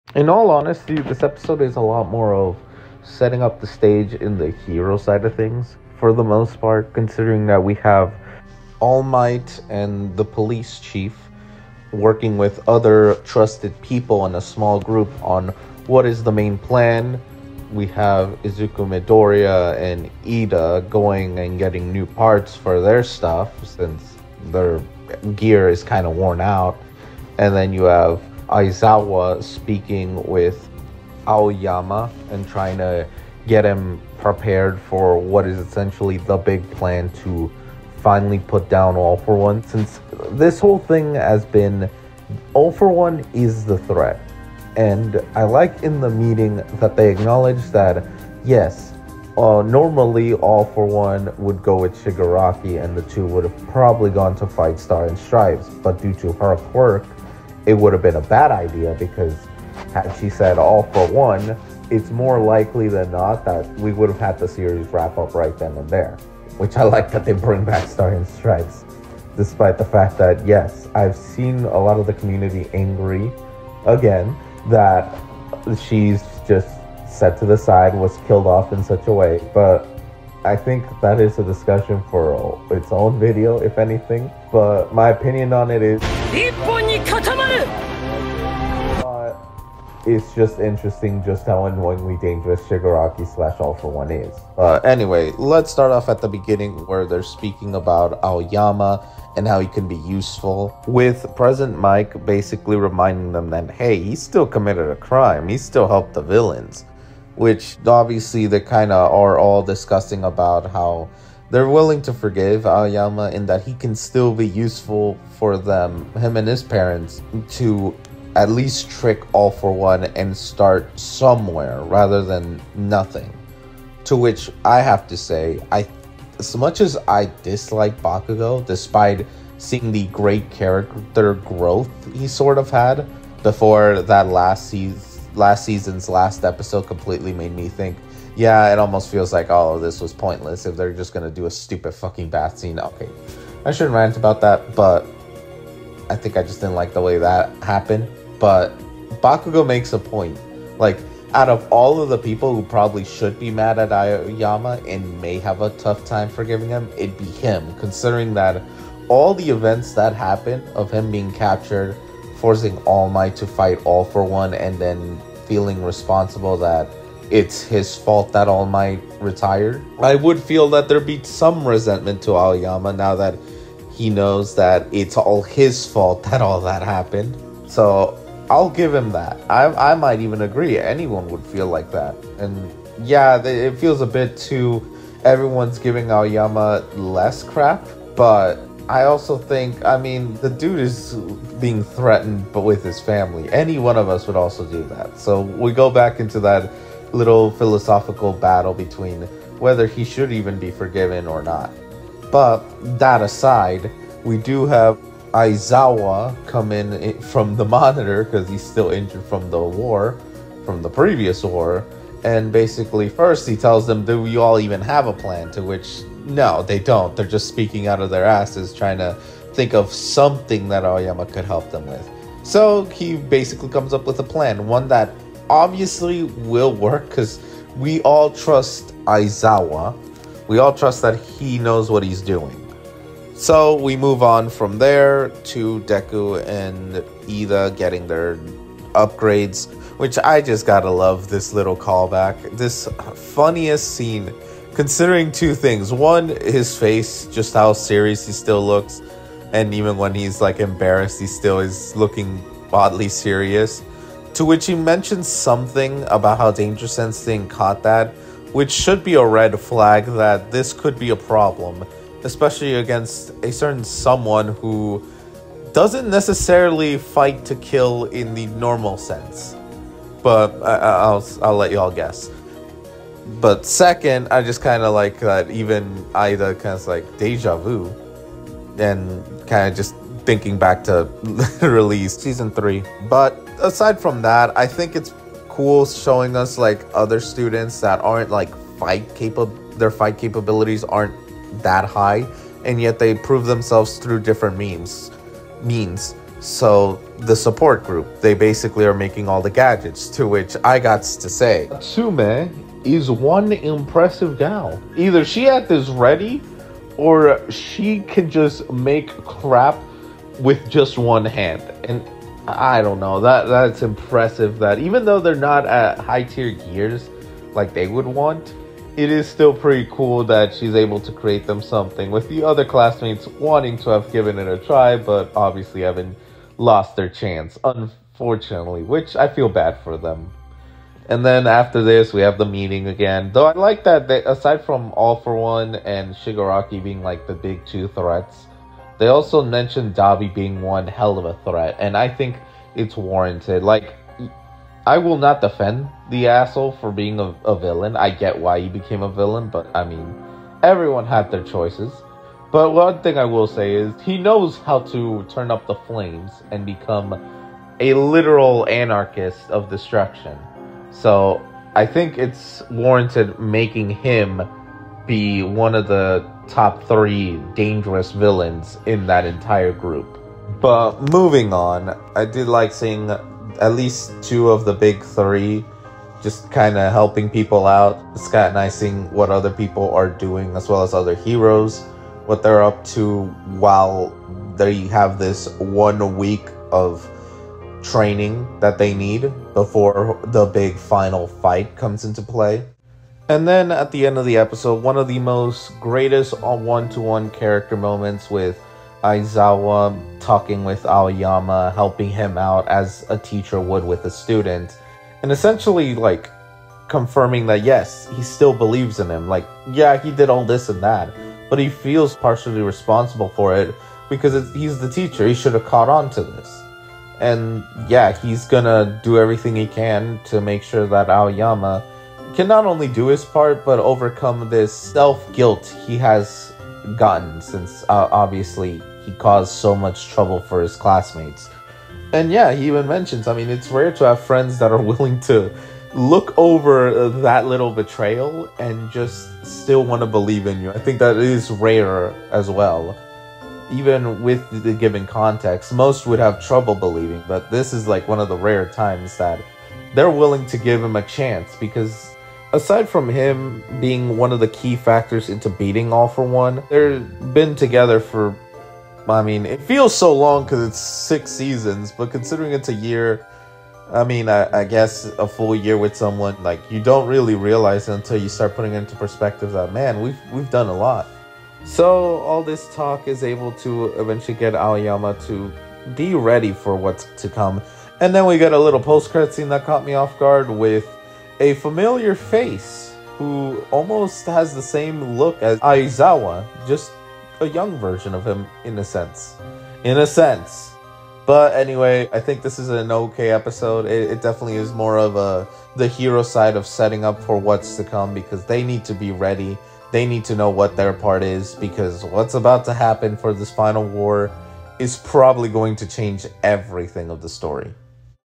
Setting up - My Hero Academia Season 7 Episode 4 Review
After the reveal of the traitor, the heroes start preparing for the big and final fight against Shigaraki, All For One, and the other villains.